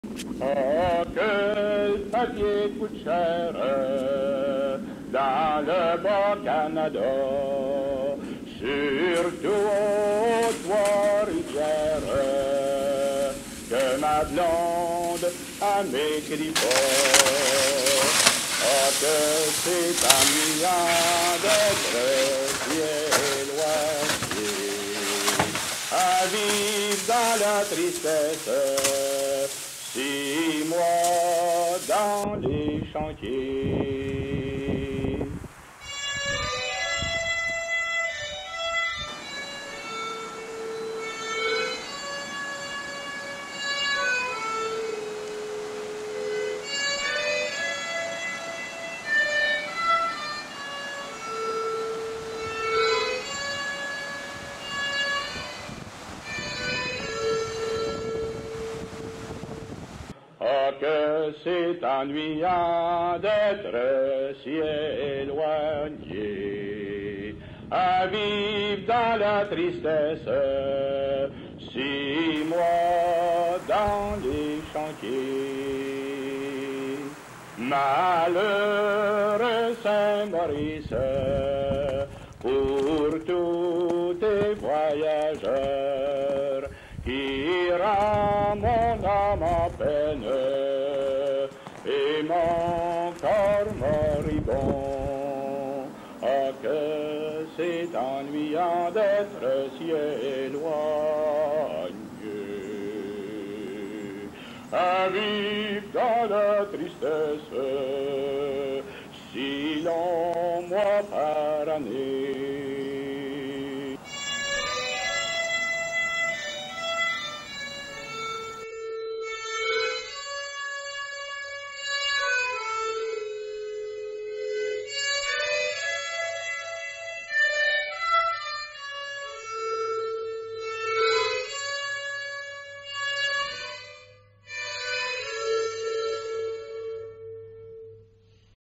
Les montagnards III
Les montagnards supportent péniblement l'isolation de l'hiver.